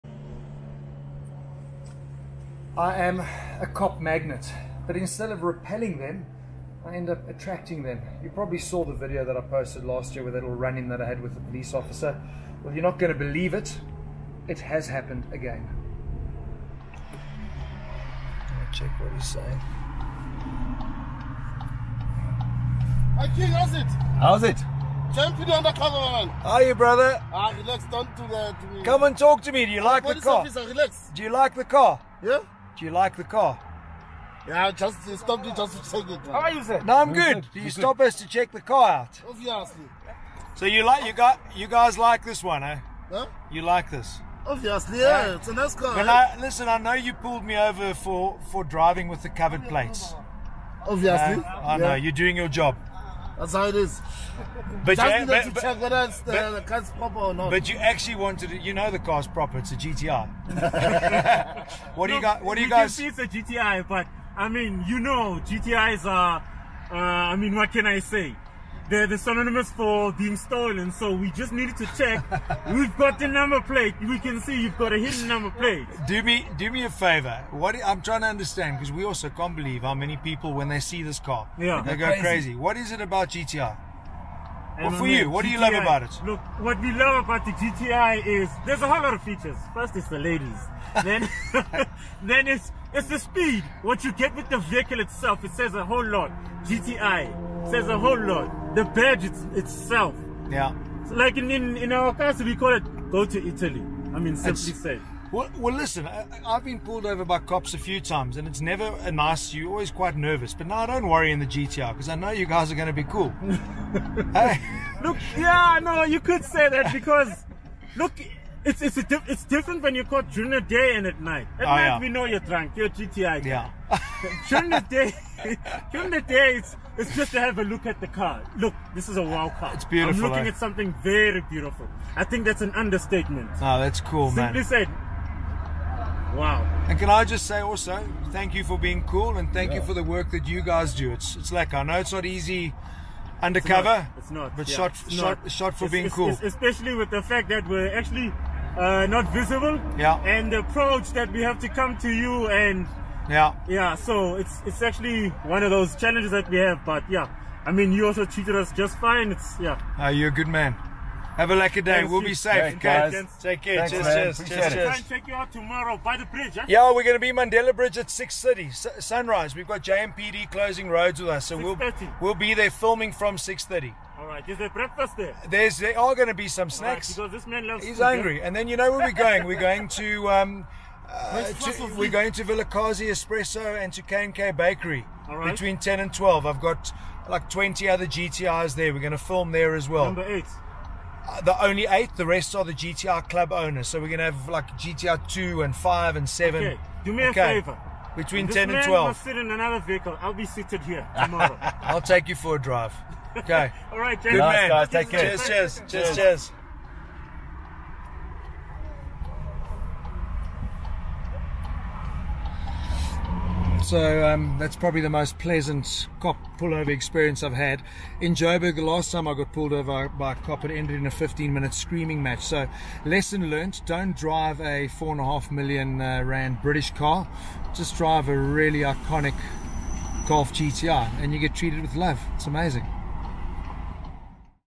COP MAGNET! Is it me or just the cars I drive???
Yep, I got pulled over AGAIN...
But I have realised I can't take it personally - its got nothing to do with me and rather everything to do with the cars I drive!

And therein lies another important lesson - what does the car you drive say about you as a person? They are extensions of our personalities, an image of ourselves we wish to portray - a statement of who we are. The thing with being a motoring journalist is that usually the cars I am driving are not my own, so its provides me with a wonderful snapshot of the psychology behind cars and how people respond to you based on what you drive.
So with my last run in with a cop, I was in an Aston Martin Vantage, this time I was behind the wheel of the Golf 8 GTI - a car that at the time that this was recorded had not yet been launched in the South African market.

I was shooting and producing content for Volkswagen for the digital launch of the car and as this was the first homologation unit in the country, it was all top secret - so we did most of our distance driving at night and did all of our filming on the scenic, quiet B-roads - when it wasn't being filmed, all the badges were taped up...

After being pulled over once 'just to see the car' I decided to have my camera ready as I was pretty sure it would happen again, such is the cult status the GTI enjoys in Mzanzi!

What a different outcome...happy days.